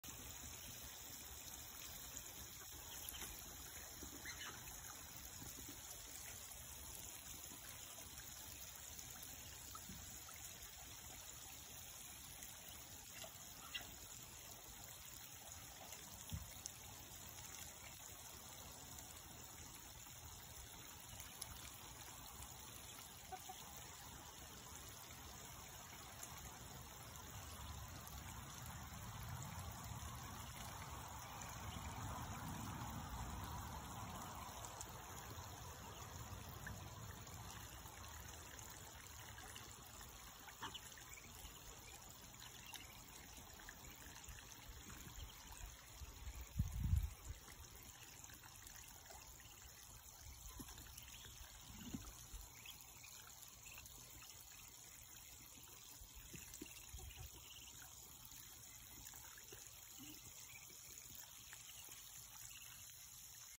Muscovy ducks enjoying some fresh water
This time of year I usually only have adults. Little Indy ￼Thought different! 10 little babies enjoying the water with the flock!